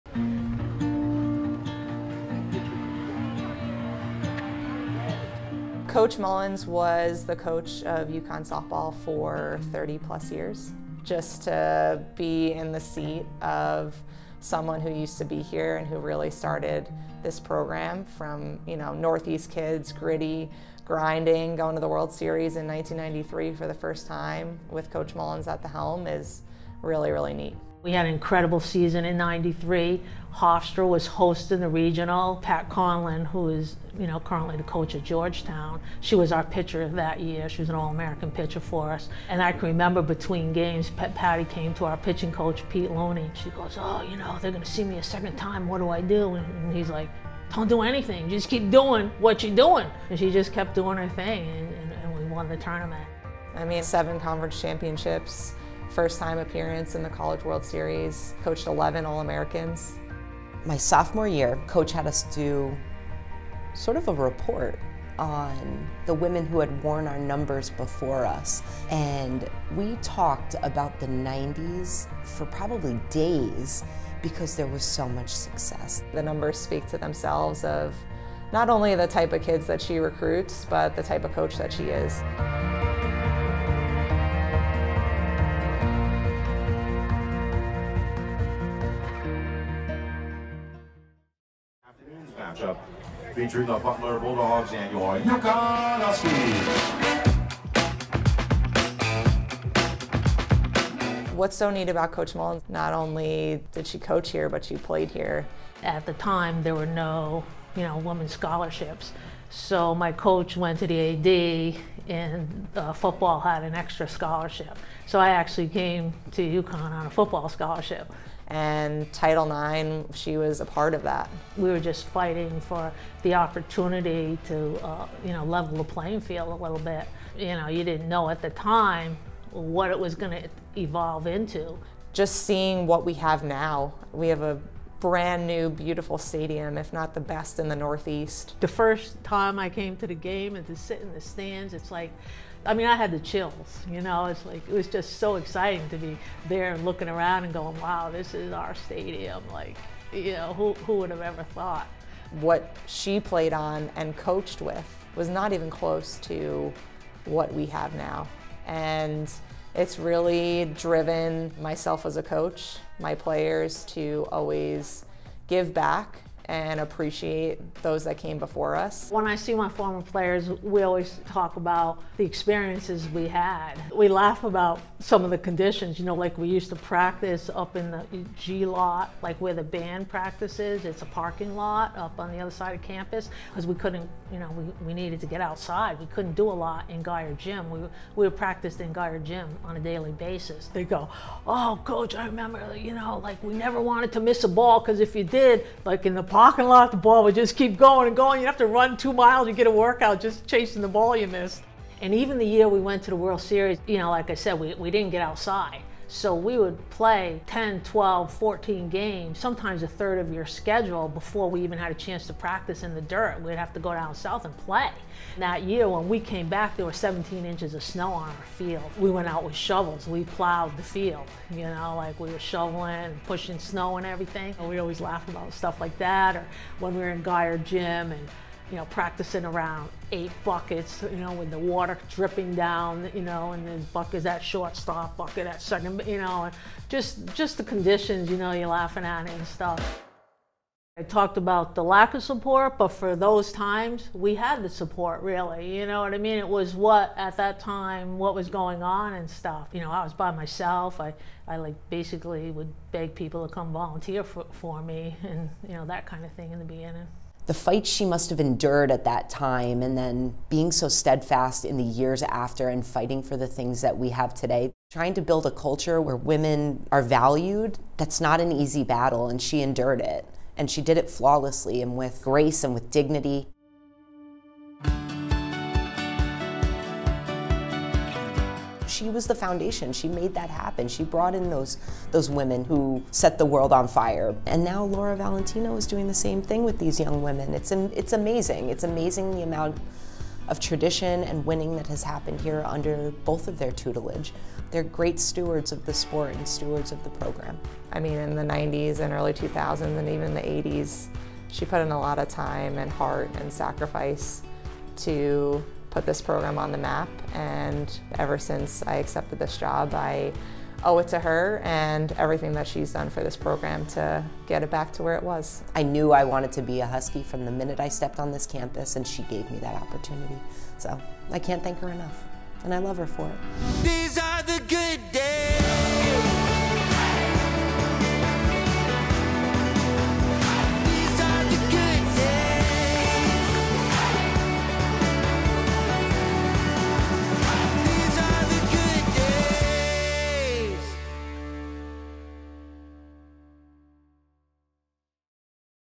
Game On: Karen Mullins | UConn Softball
Karen Mullin's career as the Head Coach of UConn Softball includes over 800 wins, 8 Big East titles, a Women's College World Series appearance, and countless student athletes developed. Hear first hand the legendary impact Coach Mullins had on UConn Athletics in episode 3 of Game On, presented by UConn Health.